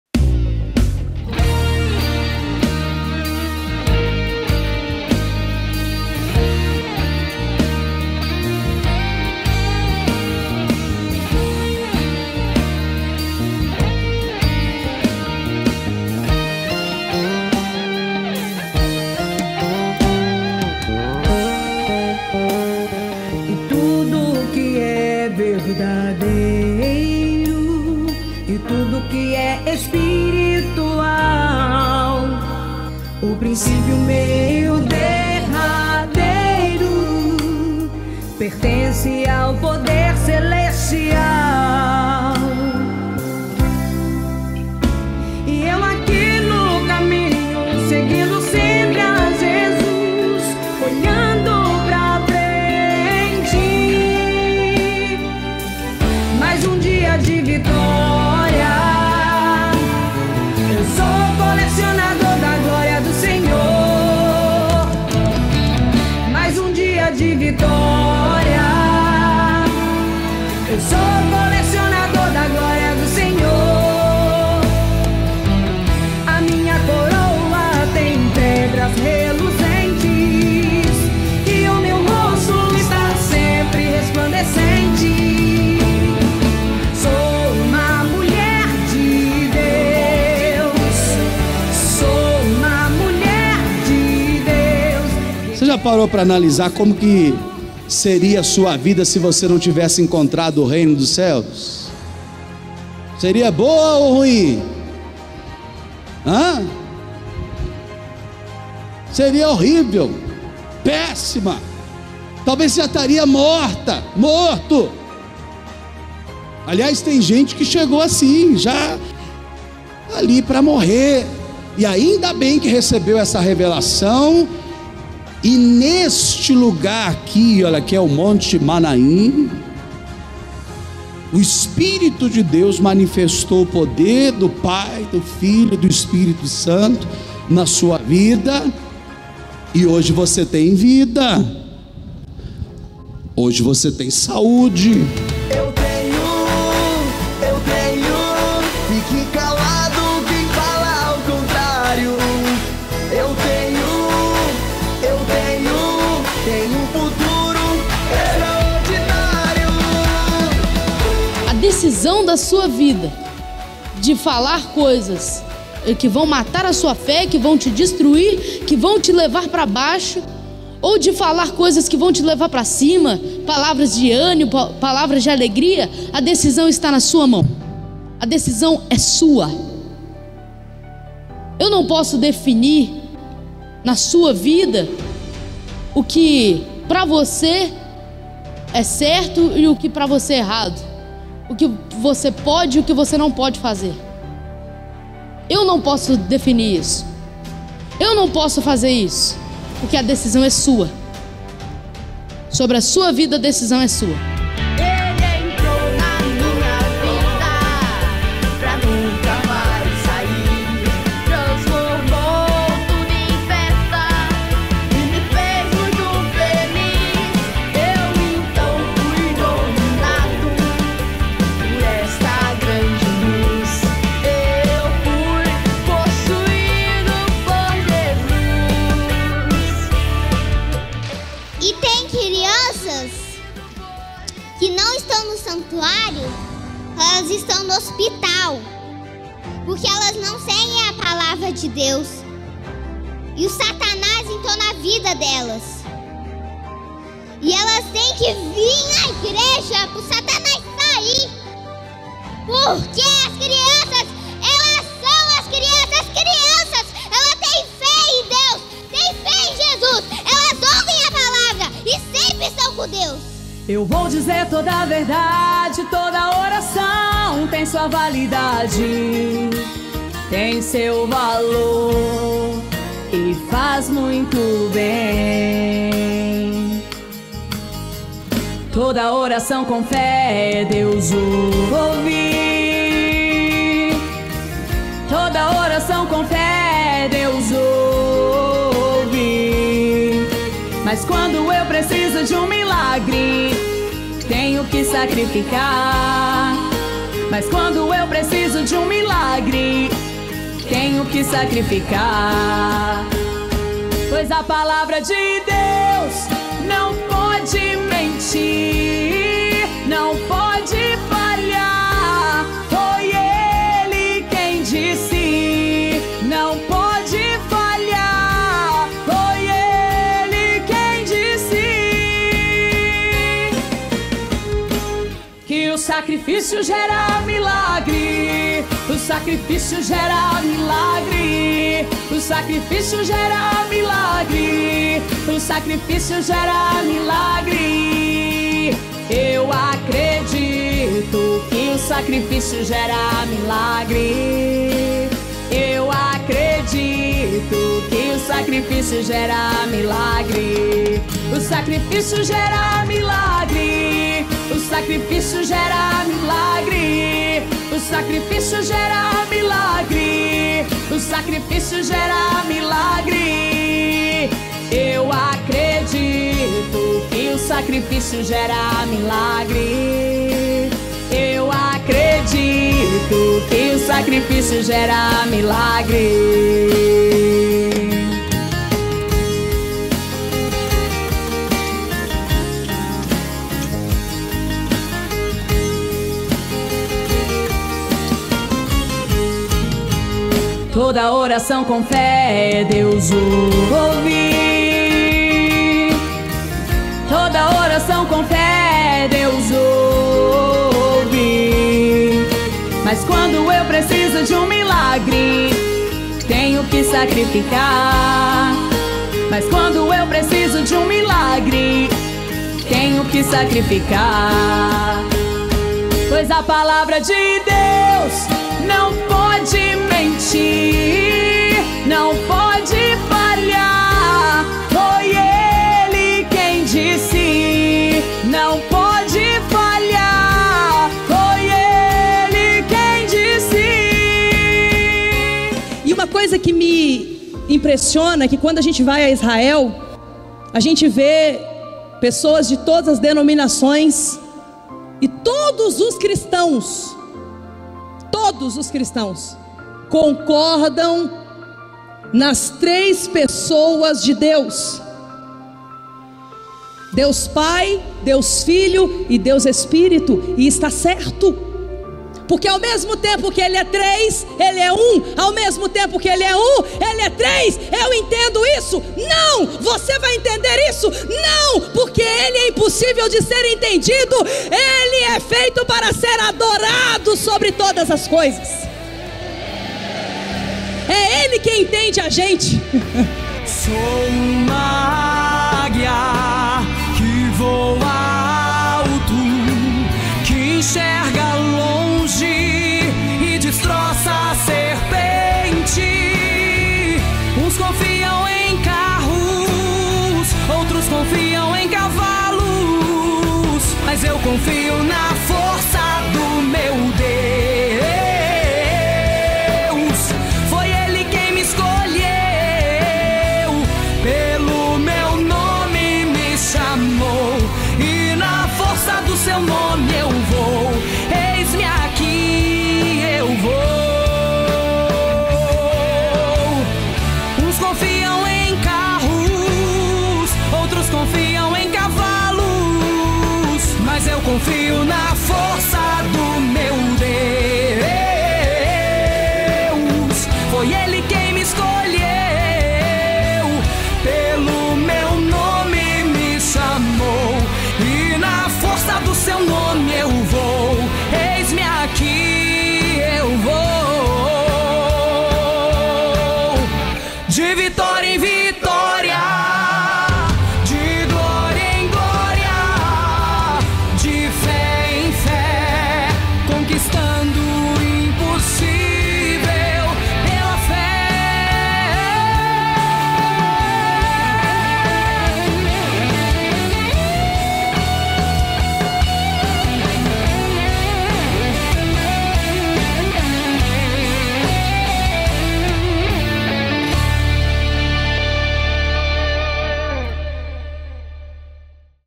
DANÇA DAS MARIAS DE MAANAIM | Junho de 2024
Confira agora um pouco de cada dança apresentada pelas nossas irmãs no Monte Maanaim!
🦅 IGREJA REINO DOS CÉUS 🦅
Uma obra forte para abalar o Brasil e o mundo!
🙏🏼 Ouça a ORAÇÃO DA MADRUGADA diariamente às 3 horas da manhã